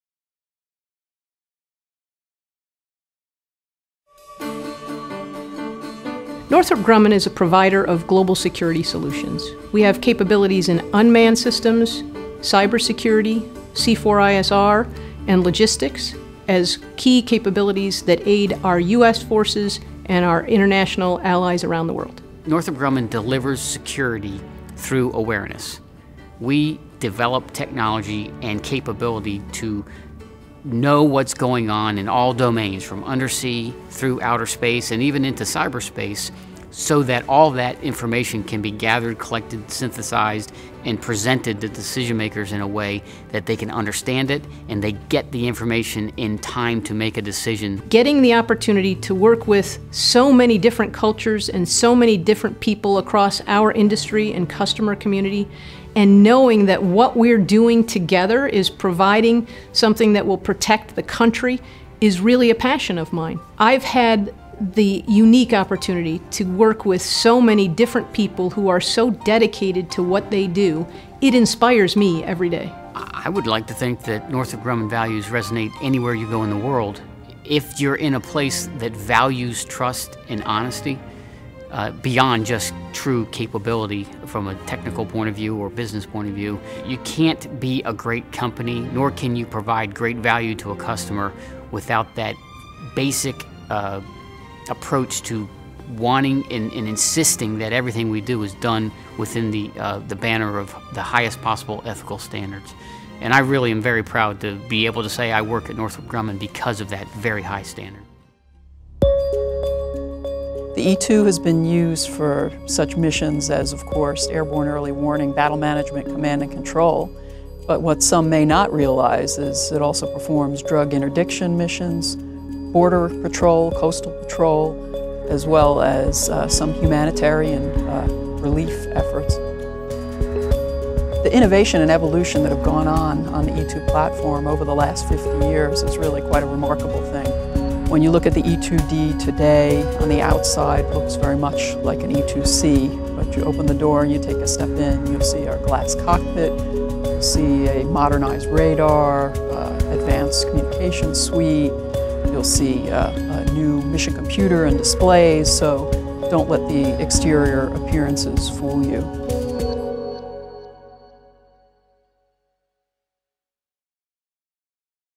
Northrop Grumman at the Dubai Airshow
The company is the prime contractor offering the E-2D Advanced Hawkeye, and provides a range of solutions for cybersecurity, C4ISR, logistics, and unmanned systems. Northrop Grumman maintains a network of offices and local businesses serving customers in Europe, the Gulf Region and Asia Pacific Regions.